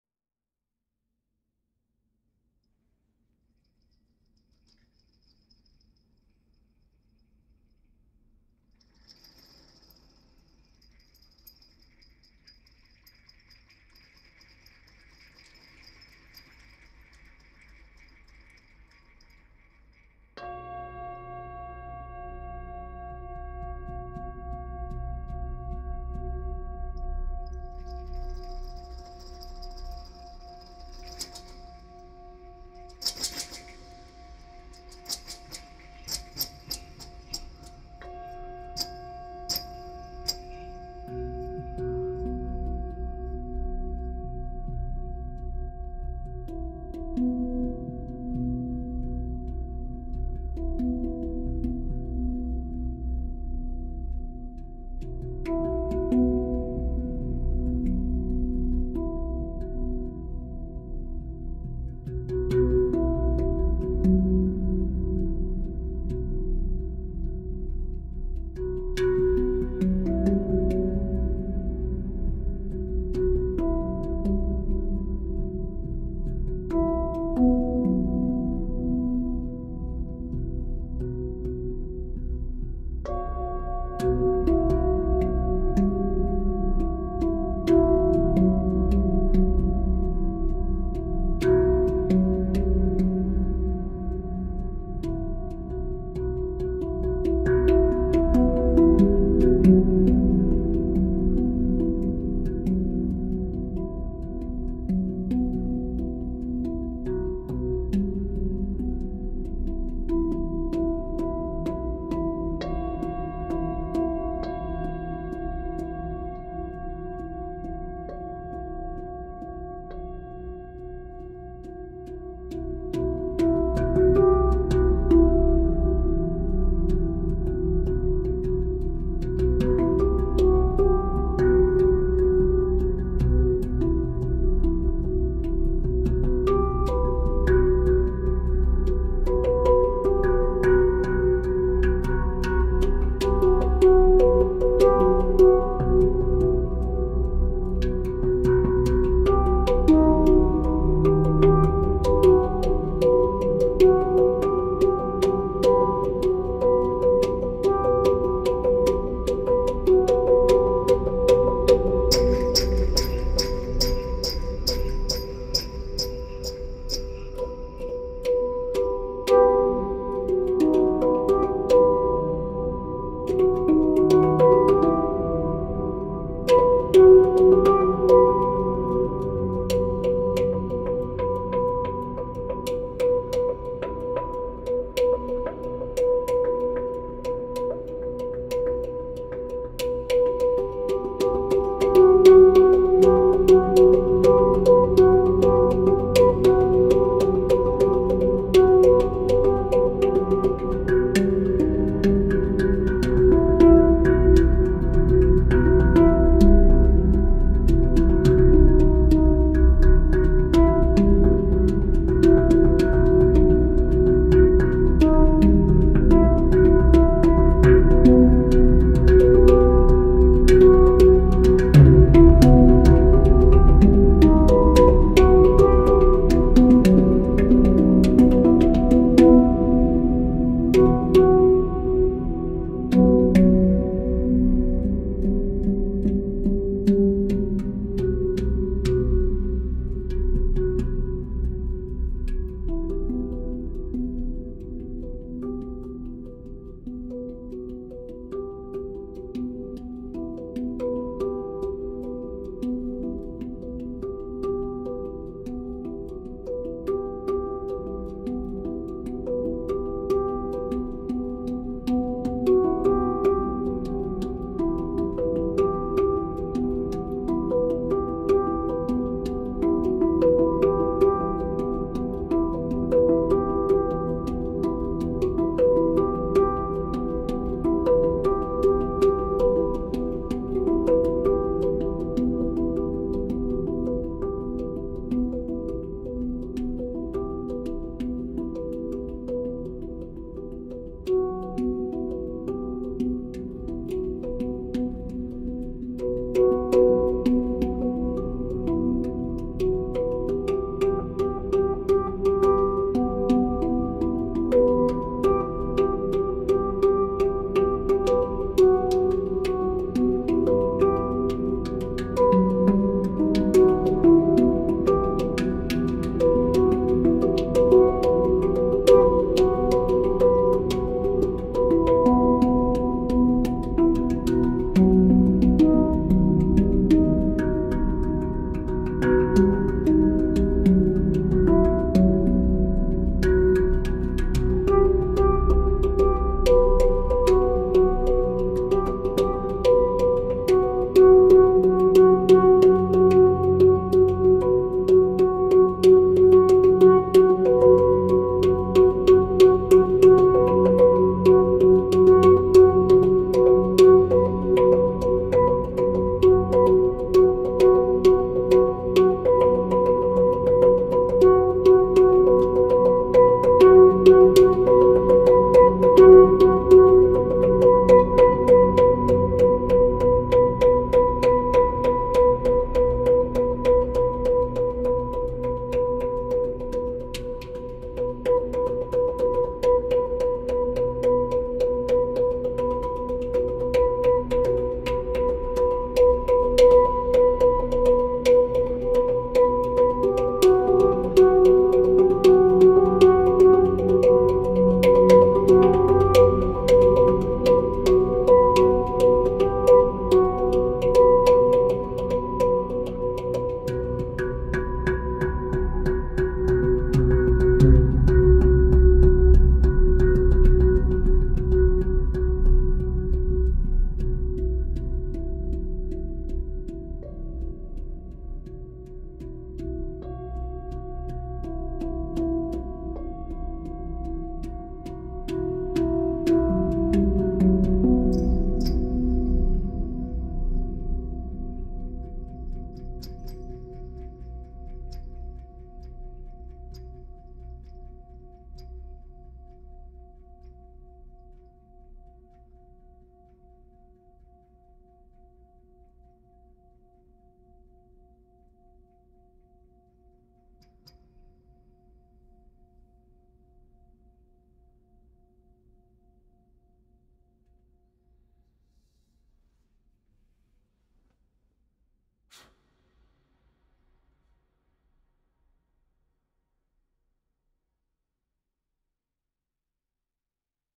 M|M DSP (mystical...or mythical...deep shamanic power!)
Shamans have been known to enter trance states and can also practice healing. We all have the power to heal in our own personal trance state. Reflection can lead to growth, let these isolated times also be a time to heal old wounds and grow into something new.

David Blink - Pantheon Steel Halo: Onoleo & Percussion
Recorded on 1/4/2021 in Mt. Shasta, CA